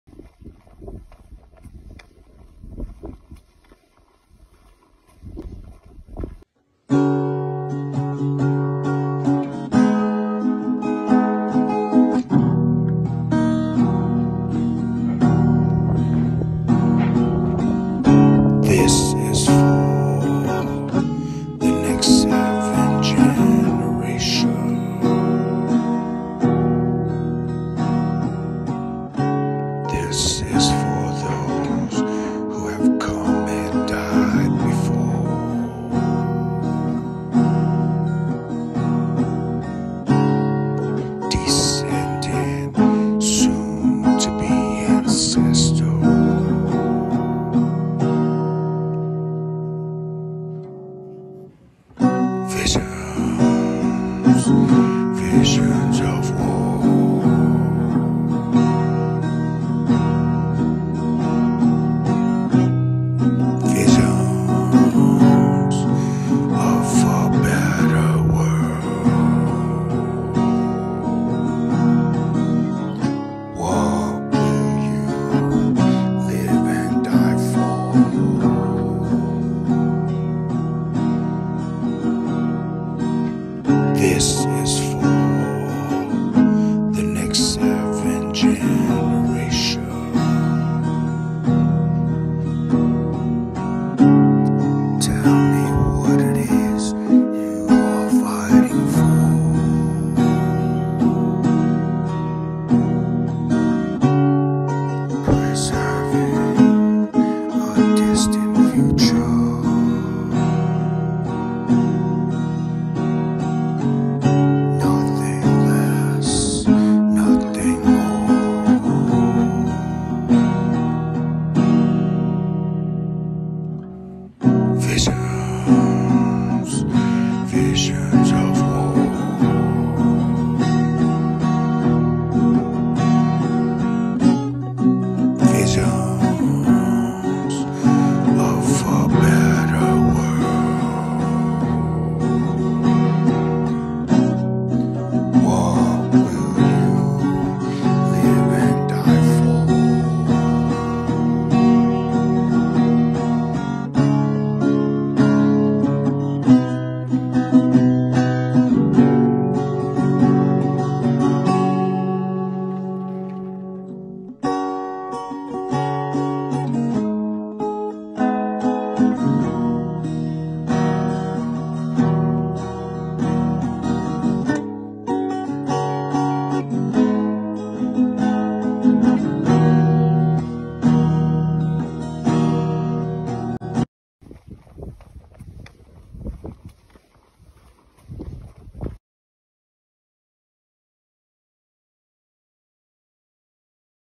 Visions of (War/ A Better World)
Original music, Spoken word poetry, footage filmed and edited by yours truly!!!
I wrote this in a few minutes and recorded myself playing it.
I’ll be releasing a bunch of improvs i recorded.